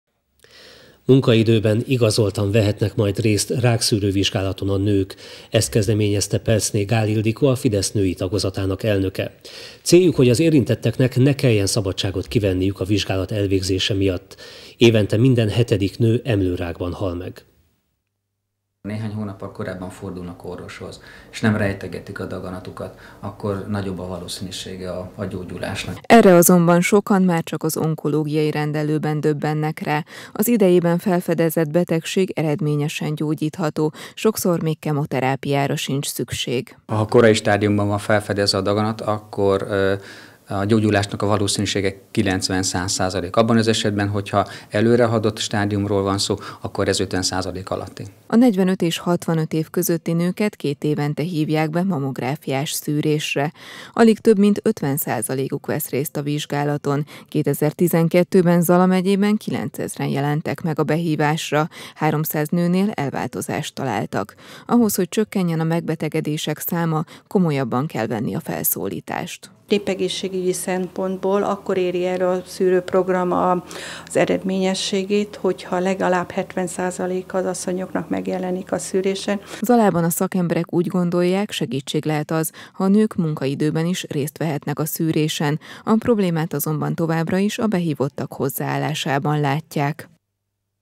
Ösztönöznék a szűrővizsgálatokon való részvételt
Munkaidőben, igazoltan vehetnek majd részt rákszűrővizsgálaton a nők. Ezt kezdeményezte Pelczné Gáll Ildikó, a Fidesz női tagozatának elnöke. Céljuk, hogy az érintetteknek ne kelljen szabadságot kivenniük a vizsgálat elvégzése miatt. Évente minden 7. nő emlőrákban hal meg.